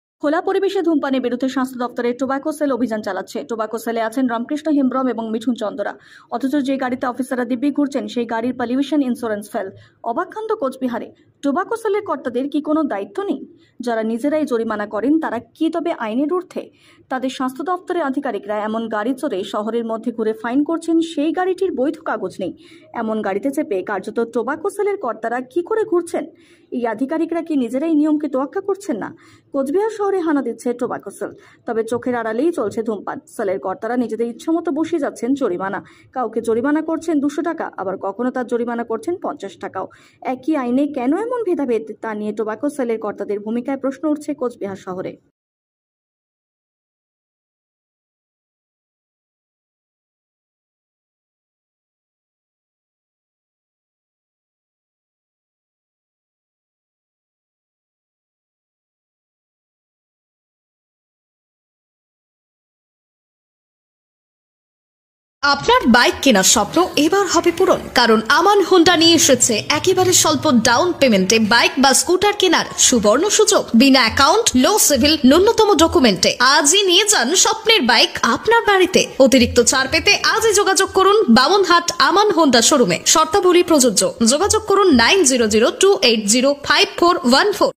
খোলা পরিবেশে ধূমপানের বিরুদ্ধে অভিযান চালানো গাড়িরই পলিউশন ইন্সুরেন্স, ট্যাক্স ফেল।
খোলা পরিবেশে ধূমপানের বিরুদ্ধে অভিযান চালানো গাড়িরই পলিউশন ইন্সুরেন্স, ট্যাক্স ফেল। অবাক কান্ড কোচবিহারে